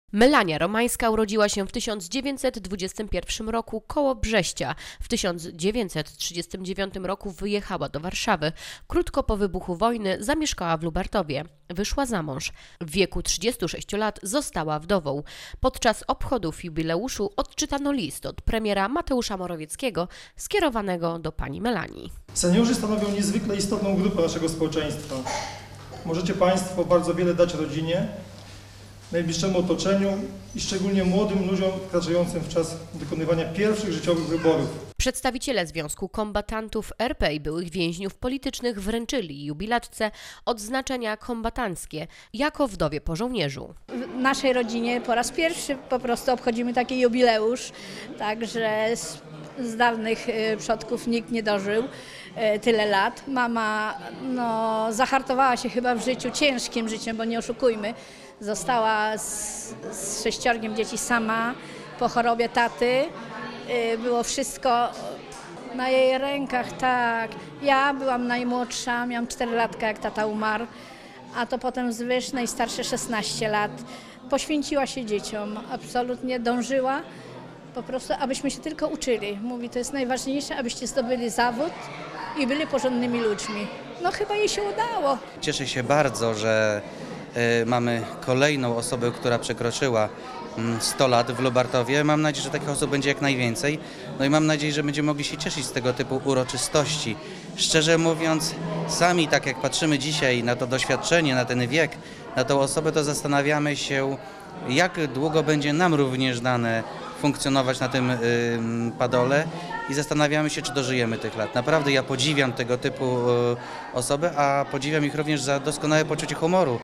Uroczystość setnych urodzin Pani Melanii Romańskiej - Telewizja Kanał S w Lubartowie
W niedziele 21 listopada zorganizowano oficjalna uroczystość z okazji 100 urodzin pani Melani Romańskiej.